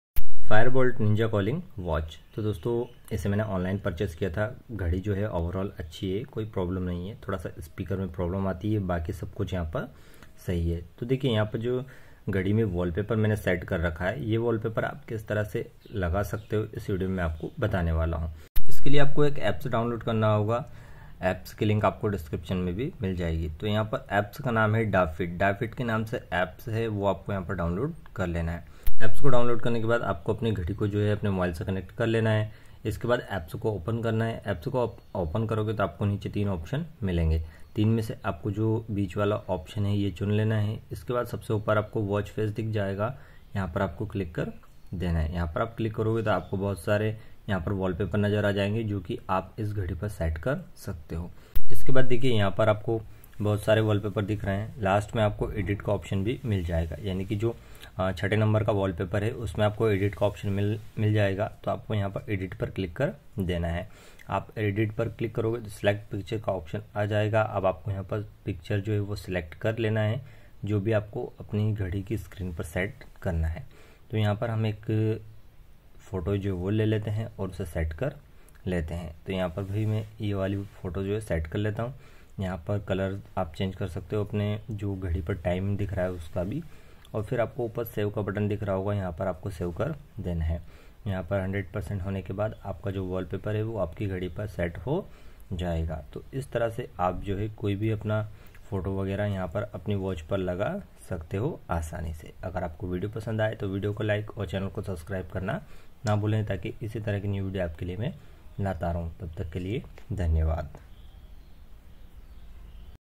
How to set wallpaper in Fire-Boltt Ninja Calling watch //smartwatch me wallpaper kaise lagaye
Fire-Boltt Ninja Calling 1.69" Full Touch Bluetooth Calling Smartwatch with 30 Sports Mode, SpO2, Heart Rate Monitoring & AI Voice Assistant (Silver), Free Size
56%
off